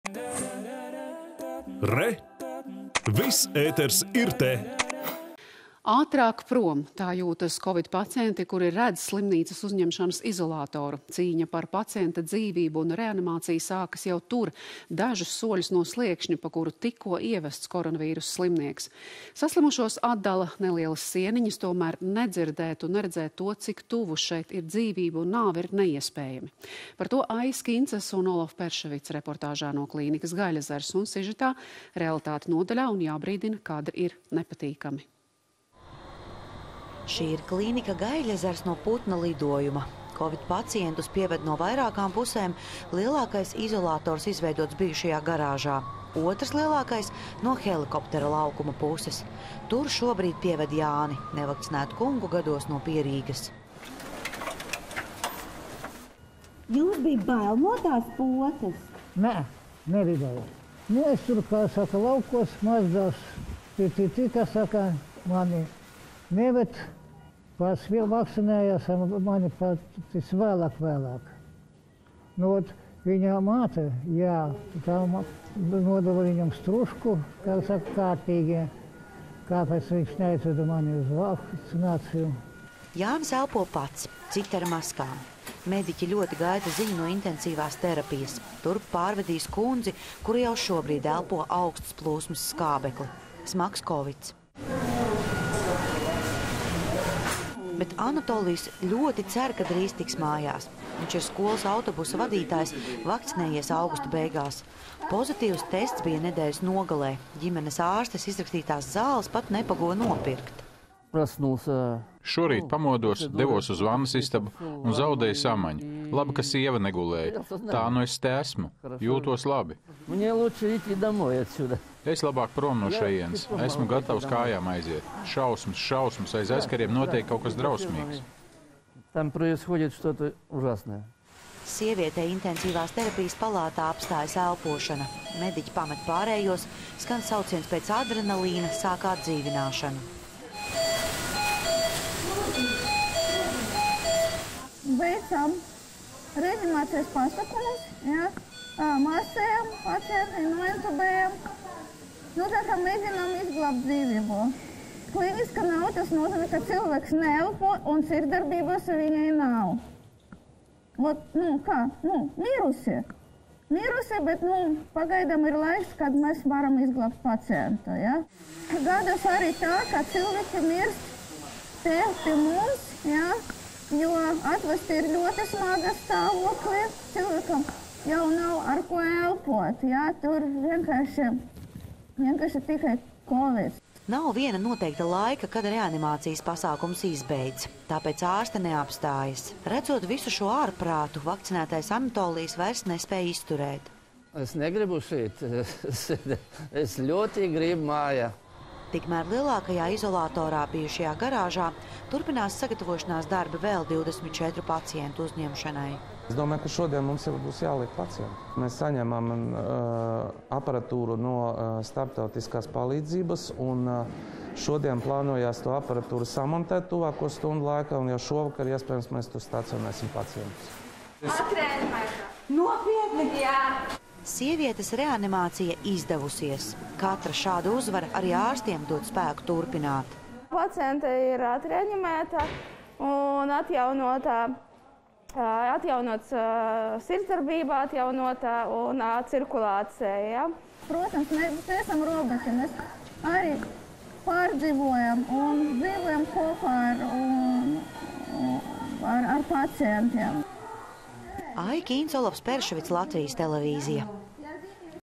Cīņa par dzīvību sākas jau uzņemšanā
Ātrāk prom! Tā jūtas Covid pacienti, kuri redz, kas notiek slimnīcas uzņemšanas izolatorā. Cīņa par pacienta dzīvību un reanimācija sākas jau tur – dažus soļus no sliekšņa, pa kuru tikko ievests koronavīrusa slimnieks. Saslimušos atdala nelielas sieniņas, tomēr nedzirdēt un neredzēt to, cik tuvu šeit ir dzīvība un nāve, ir neiespējami. Par to – Aijas Kincas un Olafa Perševica reportāžā no klīnikas Gaiļezers.  Sižetā - realitāte nodaļā un jābrīdina – kadri ir nepatīkami!